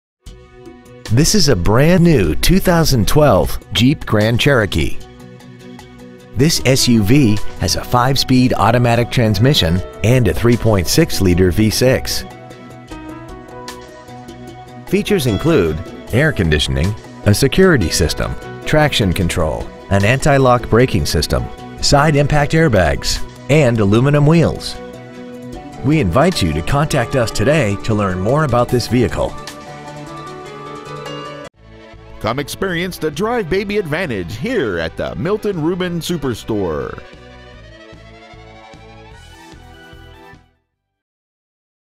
2012 Jeep Grand Cherokee Hephzibah GA
Year : 2012
 Make : Jeep
 Model : Grand Cherokee
 Engine : 3.6L V6 SFI DOHC 24V
 Trans . : Automatic
 Exterior : Brilliant Black Crystal Pearl

 Interior : Dark Graystone/Medium Graystone
 Stock : C2758

 2012 Jeep Grand Cherokee Augusta GA